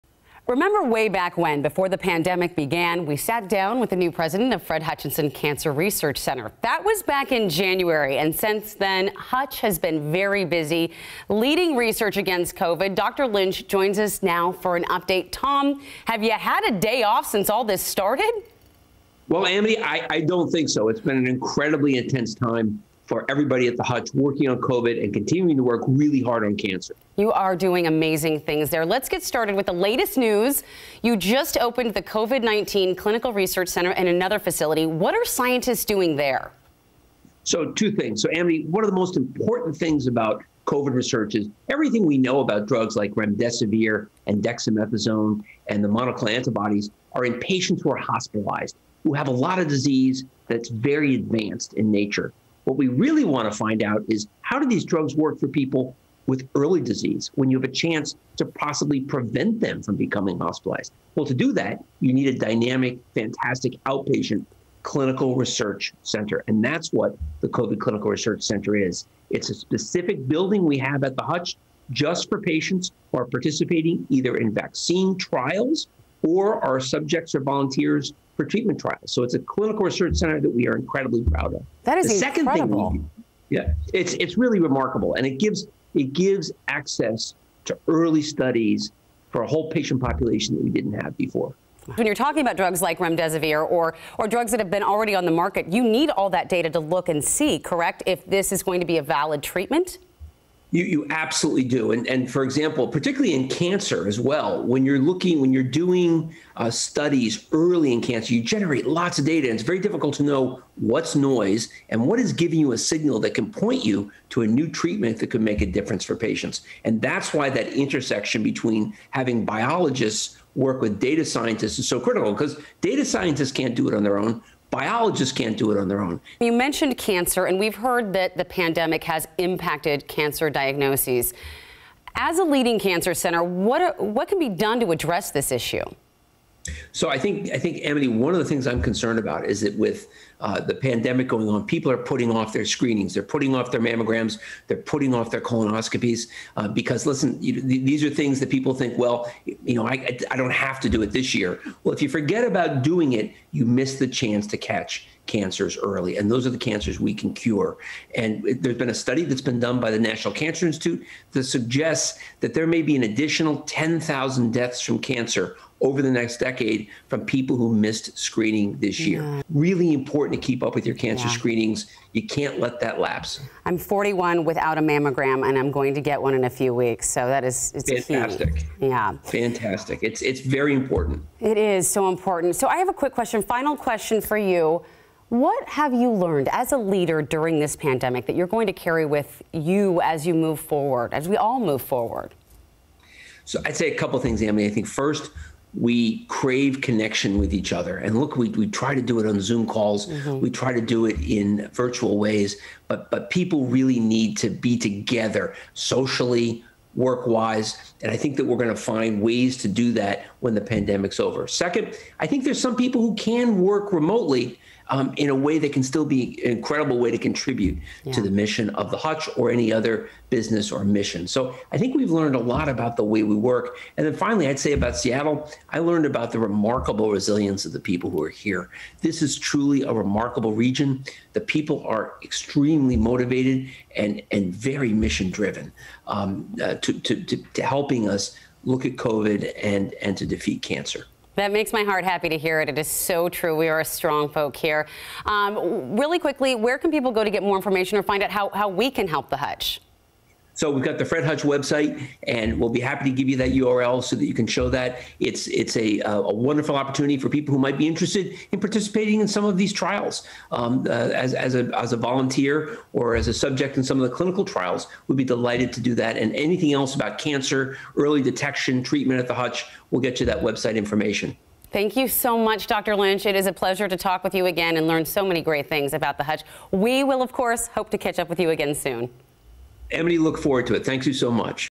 Fred Hutch opens COVID-19 clinical research center
Dr. Tom Lynch discusses the cancer research center's new COVID clinical research facility.